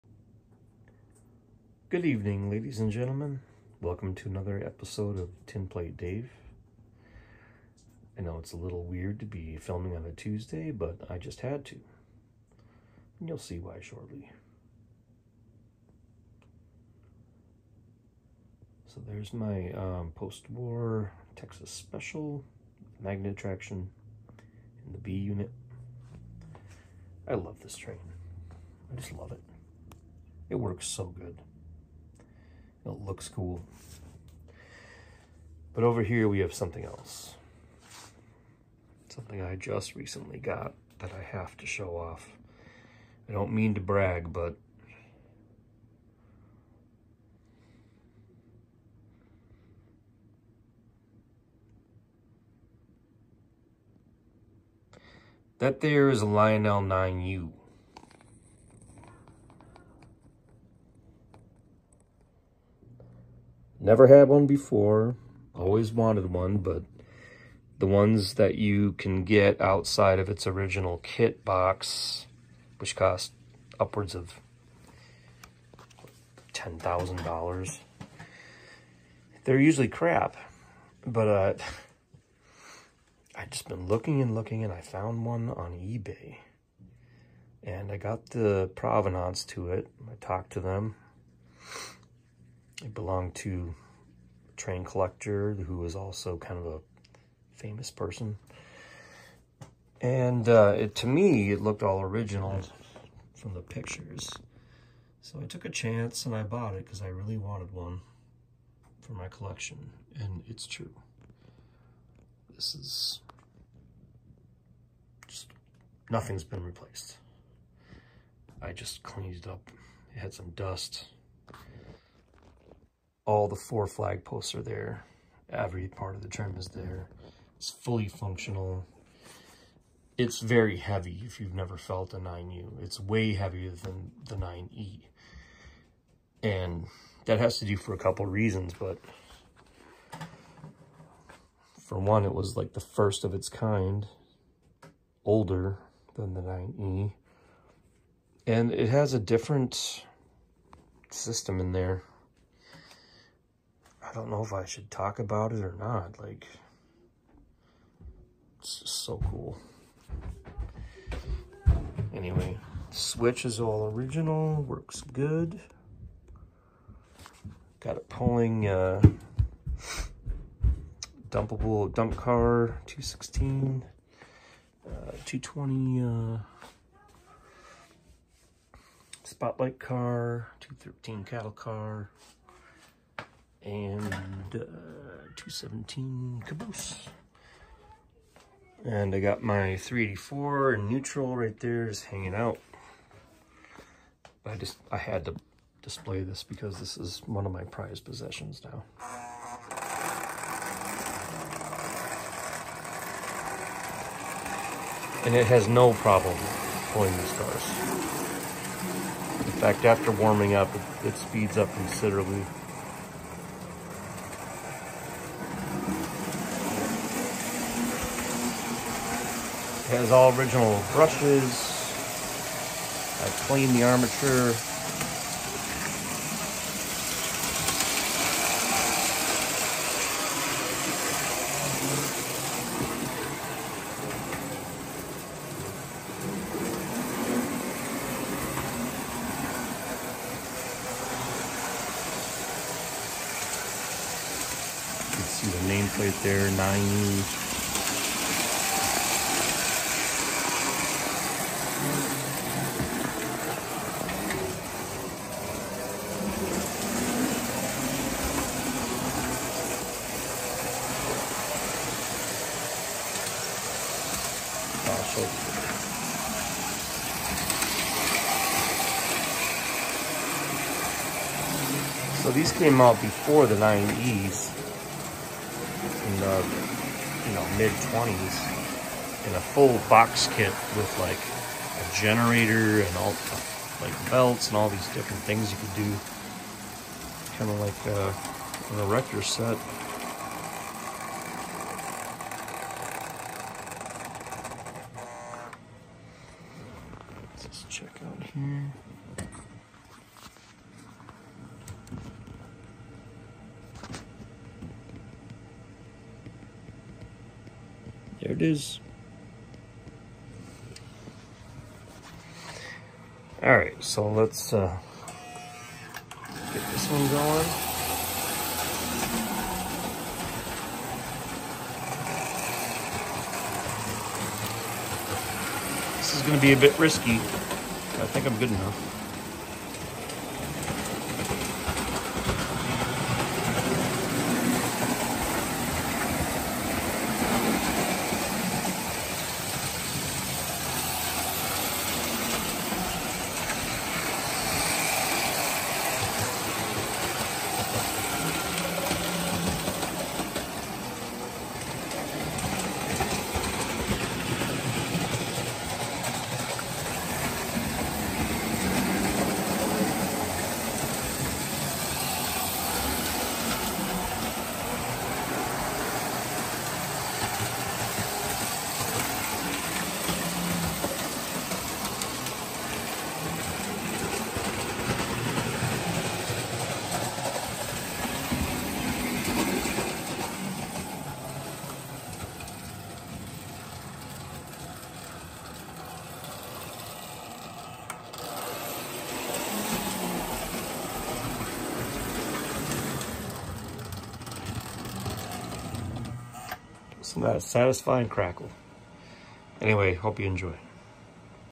Lionel Prewar 9U pulling 200 series freight cars and running along side a 1950’s Texas Special!
Original Standard Gauge 9U pulling prewar freight.  While on the O Gauge track a Texas special is running along side.